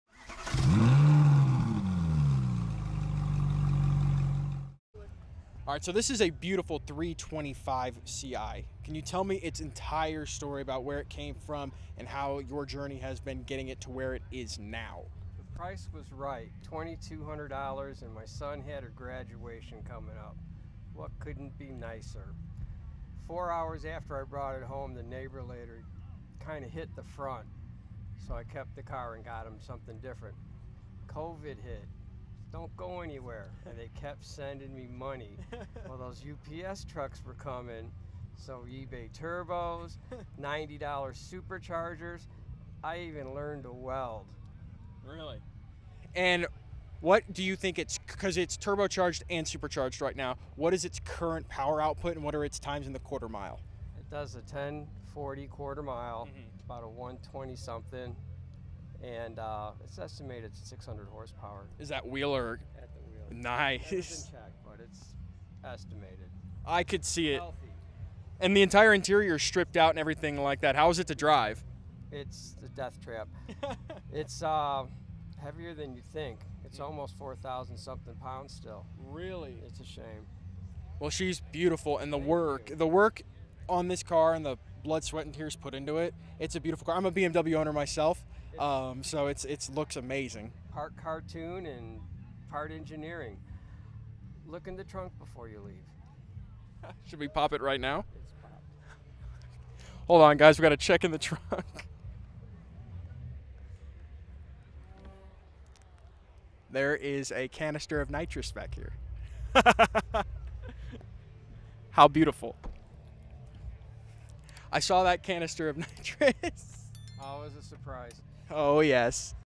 BMW 325CI Transformation
Get ready for an exciting journey through Steve's amazing BMW 325CI story. Join us from the moment he first got his hands on this car, through the challenges of COVID, and witness the thrilling transformation. See how Steve's hard work and expertise gave this 4,000-pound beast an incredible 600 horsepower boost. And don't forget to check out the trunk for a surprise - a can of nitrous, adding an extra layer of excitement to this already awesome ride. Don't miss this impressive display of automotive passion!